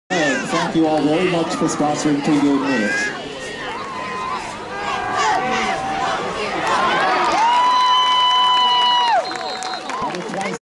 Noah Furbush Runs Down the QB for the Sack!
2012 - Kenton Wildcat Football - Week 3 - Western Buckeye League

Kenton vs Van Wert at Robinson Field

Kenton Wins 44-7!